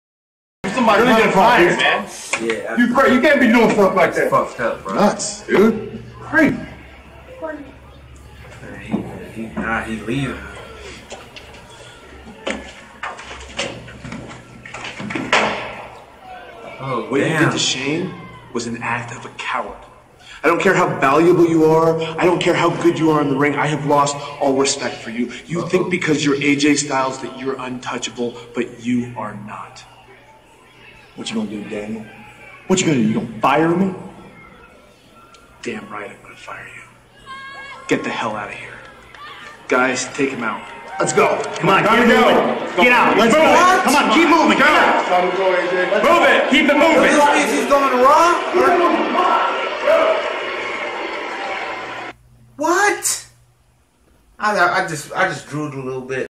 Daniel Bryan fires AJ Styles: SmackDown LIVE, March 14, 2017 Reaction
Social media-